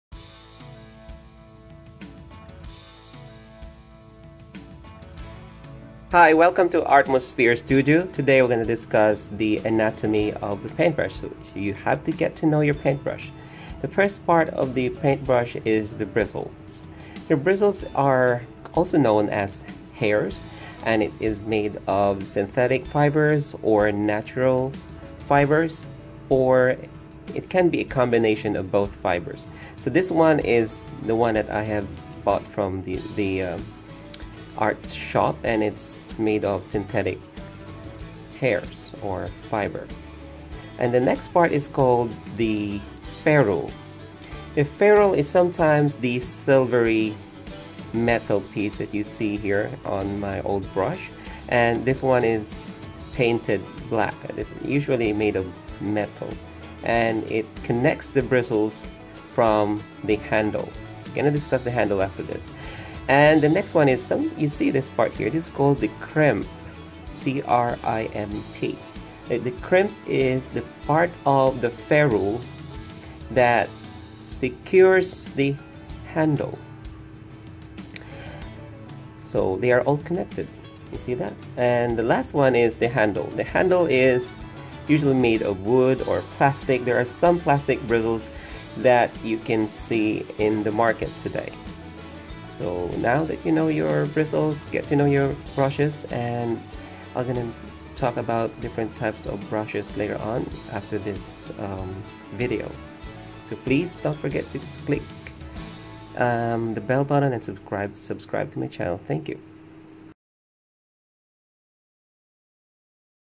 Get to know your paintbrush
In this video I will talk to you about the basic parts of a paintbrush!